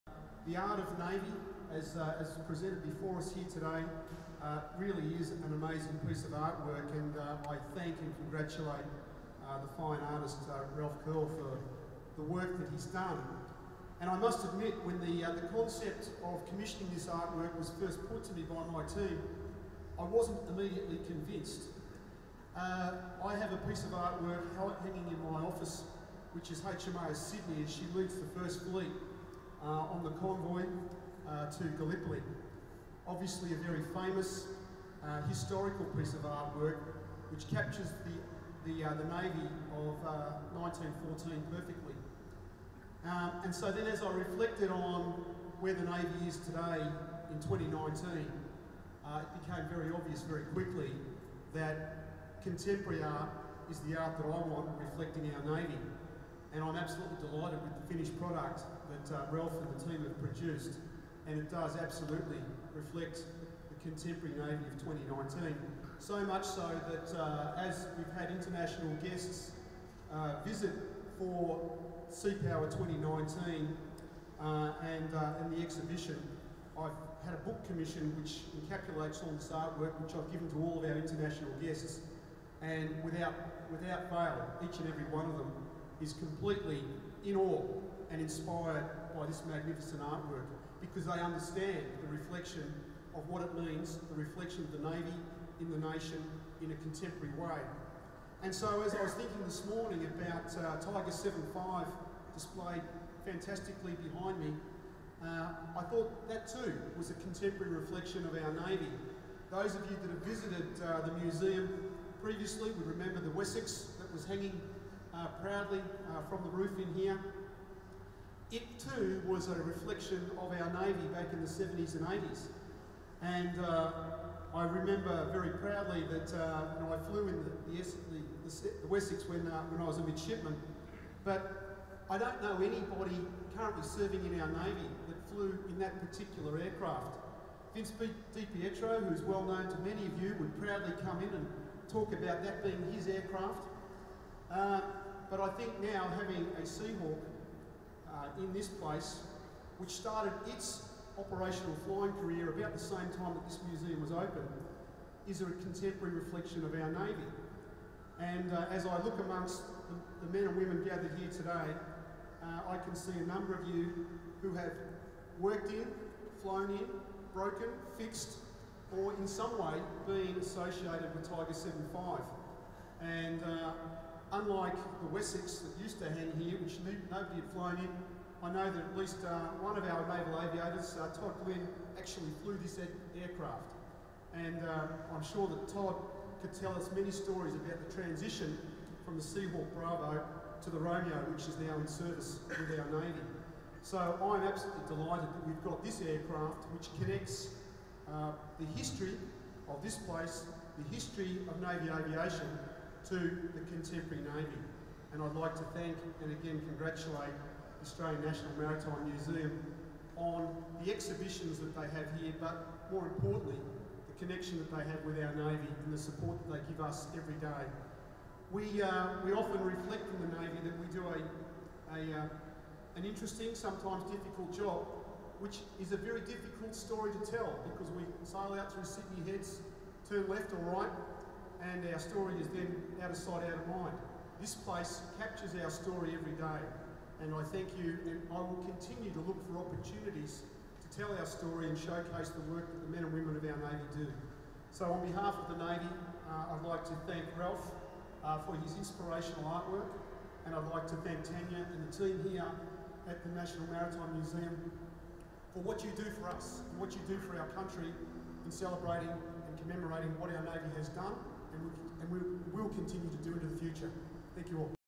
The Launch of the Art of Navy Exhibition by Vice Admiral Michael Noonan, Chief of Navy, Australia
The speech given by Admiral Noonan at the opening of the Art of Navy exhibition at the Australian National Maritime Museum as part of the Sea Power Conference, Sydney Australia October 2019